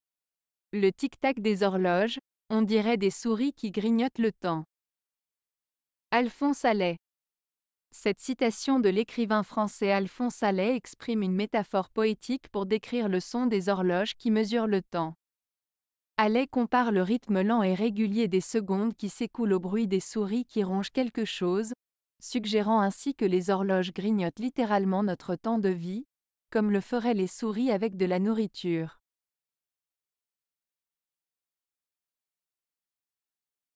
Le tic-tac des horloges, on dirait des souris qui grignotent le temps. - Alphonse Allais
Cette citation de l'écrivain français Alphonse Allais exprime une métaphore poétique pour décrire le son des horloges qui mesure le temps. Allais compare le rythme lent et régulier des secondes qui s'écoulent au bruit des souris qui rongent quelque chose, suggérant ainsi que les horloges grignotent littéralement notre temps de vie, comme le feraient les souris avec de la nourriture.